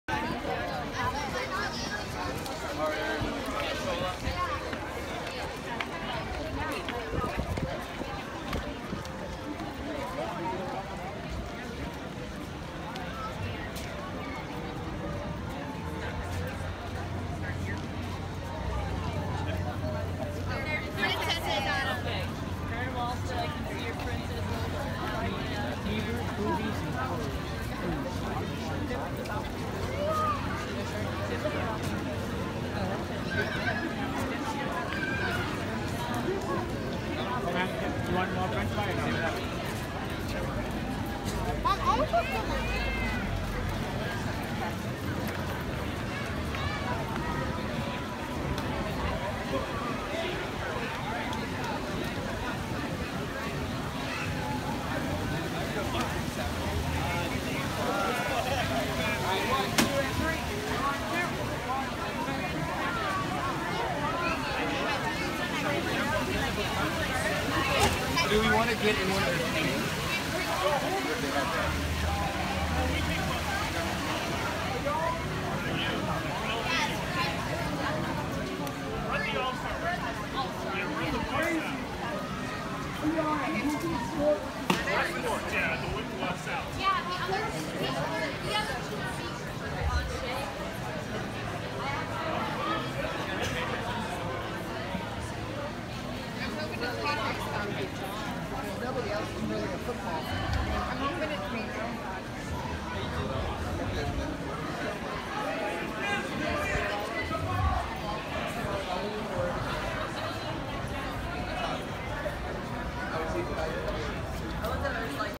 France Pavilion - Epcot Centre
Quick walk through of the France Pavilion at Disney's Epcot Centre on a busy Saturday evening.